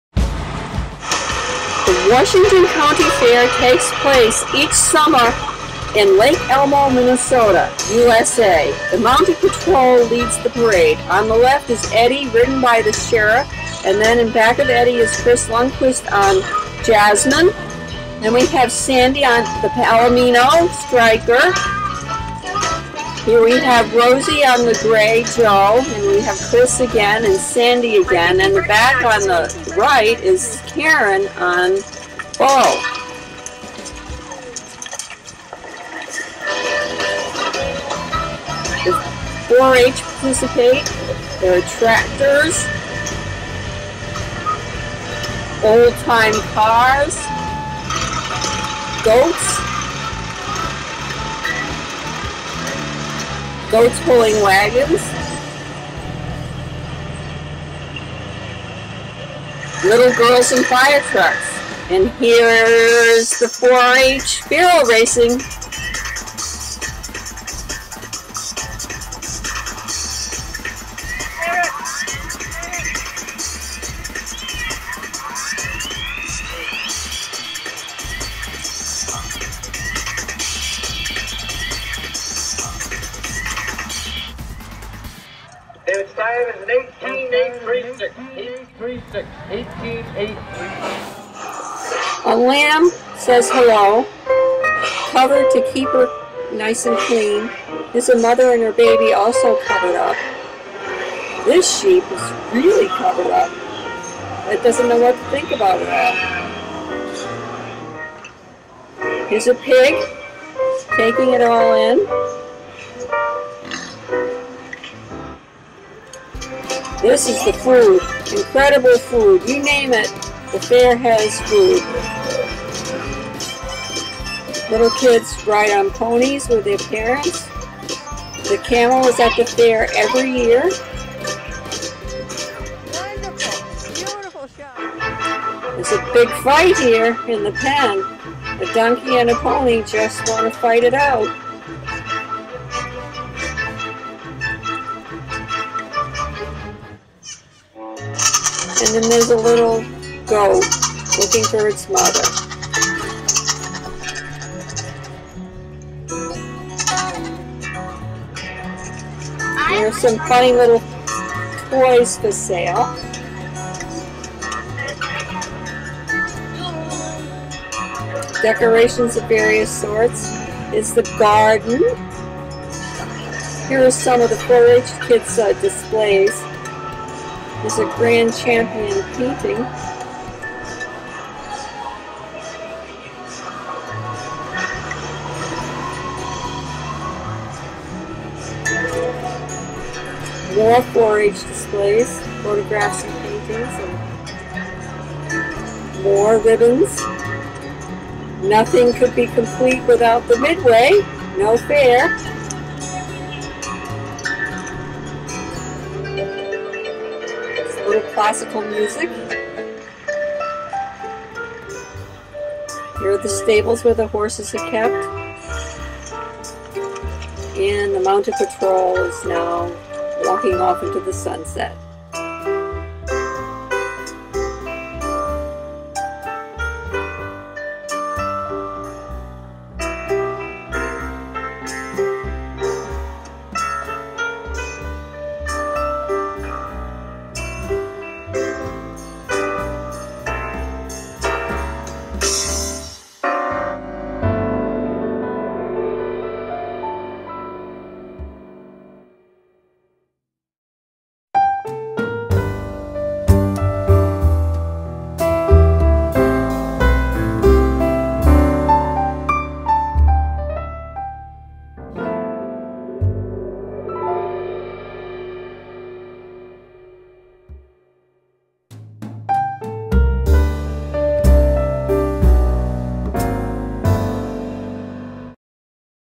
Washington County Fair 2009
The Washington County Fair in Lake Elmo,  Minnesota, is an exciting place to be. The fair begins with a parade that the Mounted Patrol leads. Goat drawn wagons, antique cars, and and 4-H floats are part of the parade. The fair has horse events of all kinds, such as barrel racing, Western pleasure, and hunt seat equitation.  Sheep, goats, pigs, chickens, cows, and horses are on show. Deep fried pickles, pronto pups, and cheese curds are yours if you want them.  Music and dancing, pony rides, camel rides, and displays of flowers, photographs, paintings, and other creative activities are there, too. A county fair is a great place to be.